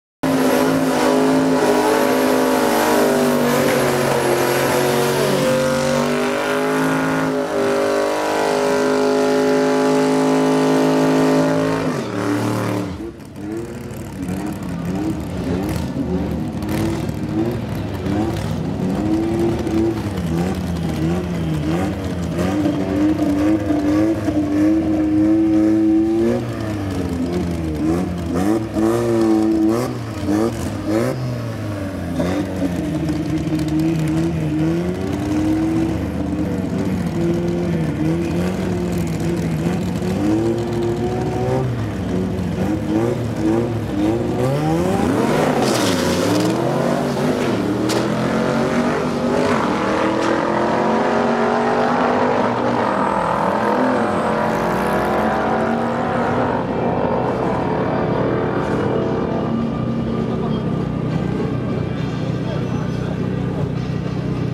April 2013 QTM Day 3 - Run 04
13th April 2013 - Day 3 of the First Quarter Mile Event of the 2013 Championship held at Hal Far Raceway